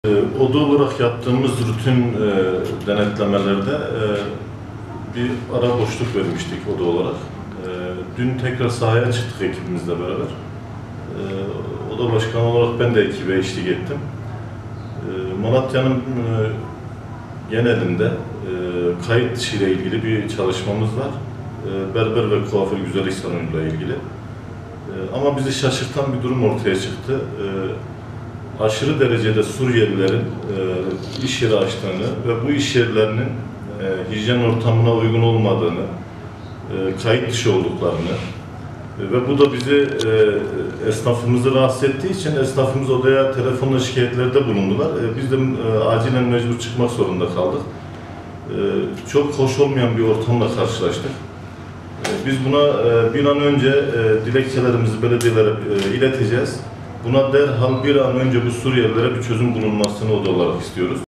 Denetimler artırılsın
Malatya Berberler ve Kuaförler Odası Başkanı Ramazan Toptaş, oda olarak il genelinde yaptıkları denetimler sonucunda çok sayıda kaçak Suriyeli berber tespit ettiklerini belirterek, belediyelerin denetim yapması gerektiğini söyledi. Toptaş, “Hiçbirinin ne vergi levhası ne de bir belgesi var. Hepsi kaçak” dedi.